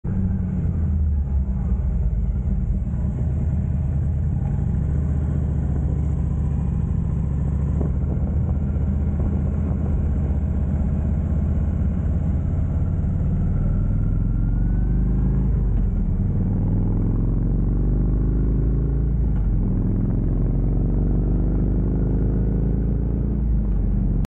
Not many more days like this.
Getting out for a ride while we still can before winter.